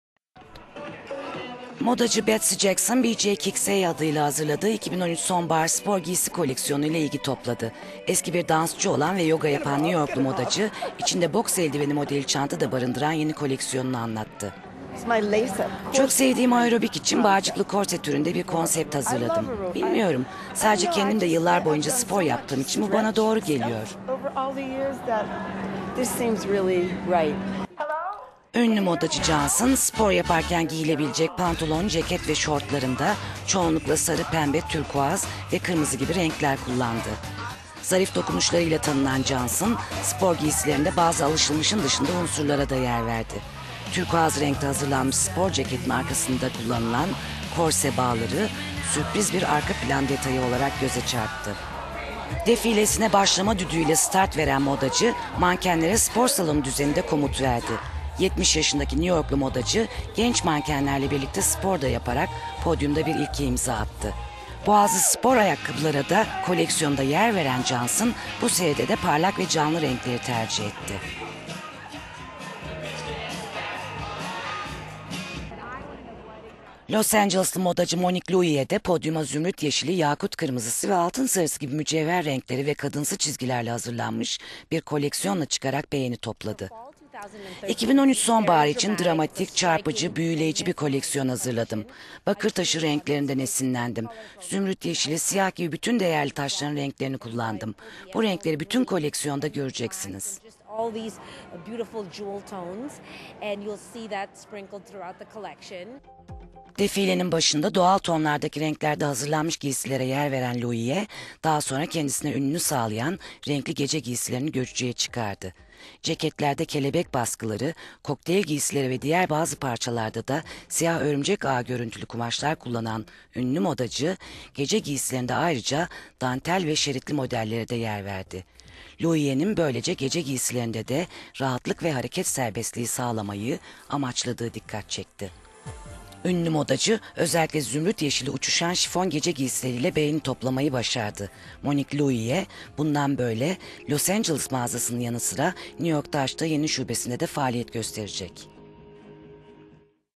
euronews le mag - New York'ta moda rüzgarı esmeye devam ediyor
Hazırladığı ''BJ kicks a'' adlı spor giysi koleksiyonuyla New york Moda Haftası'na katılan Modacı Betsy Johnson, giysilerinin yanısıra podyum şovuyla da ilgi topladı.

Eski bir dansçı olan ve yoga yapan New York'lu  modacı, spor yaparken giyilebilecek pantolon, ceket ve şortlarında  çoğunlukla sarı, pembe, turkuaz ve kırmızı gibi parlak renkler kullandı.

Zarif dokunuşlarıyla tanınan Johnson, spor giysilerinde bazı alışılmışın dışında unsurlara da yer verdi. Turkuaz renkte hazırlanmış spor ceketin arkasında kullanılan korse bağları ''sürpriz bir arka plan detayı'' olarak göze çarptı.

Defilesine başlama düdüğüyle start veren modacı, mankenlere spor salonu düzeninde komut verdi. 70 yaşındaki New York'lu modacı genç mankenlerle birlikte spor da yaparak podyumda bir ilke daha imza attı.

Monique Lhuillier de podyuma zümrüt yeşili, yakut kırmızısı ve altın sarısı gibi mücevher renkleri ve kadınsı çizgilerle hazırlanmış bir koleksiyonla çıkarak beğeni topladı.

''2013 sonbaharı için dramatik, çarpıcı ve büyüleyici bir koleksiyon hazırladım. Bakırtaşı'nın renklerinden esinlendim. Zümrüt yeşili, siyah gibi bütün değerli taşların renklerini kullandım. Bu renkleri bütün koleksiyonda göreceksiniz '' diyen modacı, renkli gece giysileriyle ilgi çekti.

Ceketlerde kelebek baskıları, kokteyl giysileri ve diğer bazı parçalarda da siyah örümcek ağı görüntülü kumaşlar kullanan ünlü modacı, gece giysilerinde ayrıca dantel ve şeritli modellere de yer verdi.